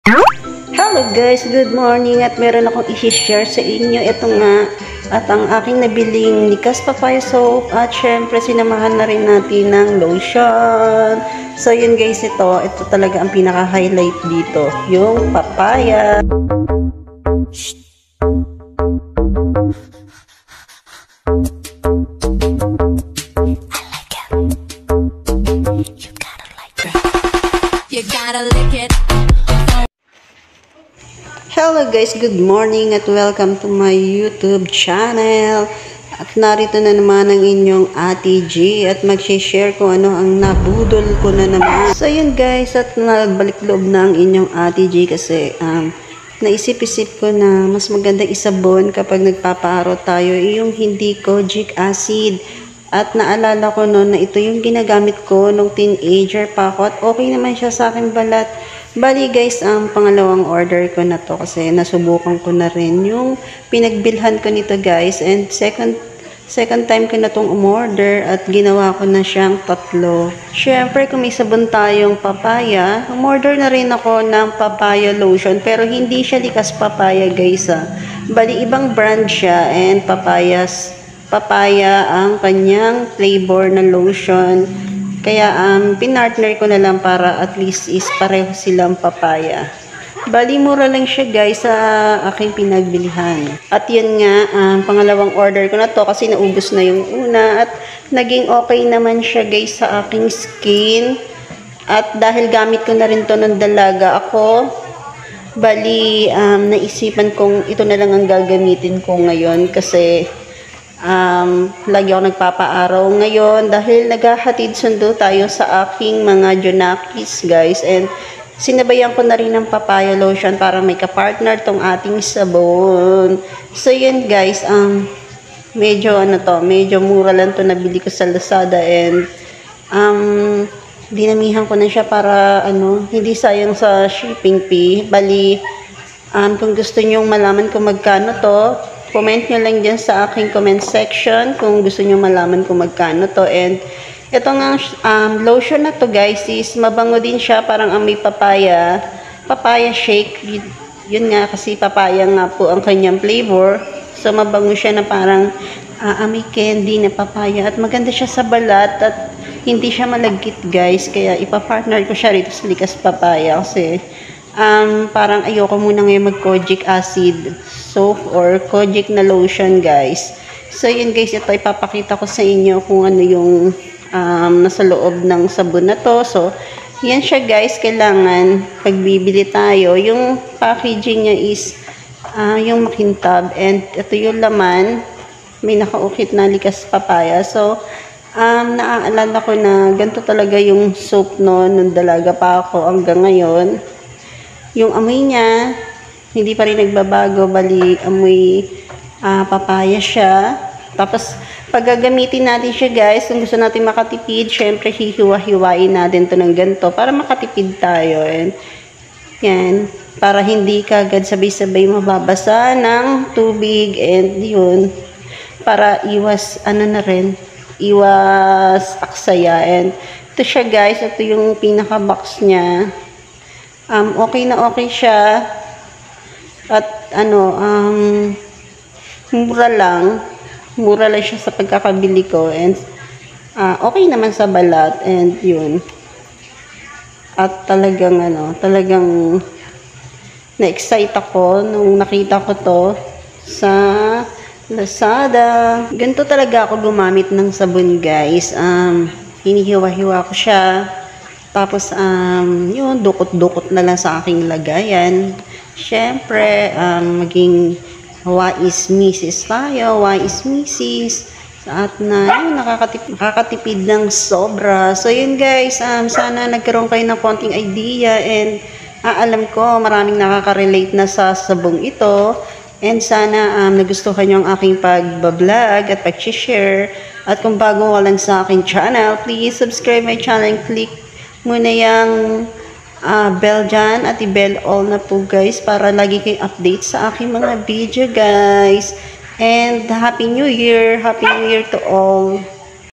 LIKAS PAPAYA SOAP
Likas papaya Ang ginagamit ko ngayon Hindi muna ako mag sasabon Ng Kojic
Likas papaya maganda sa balat mula noon Hanggang ngayon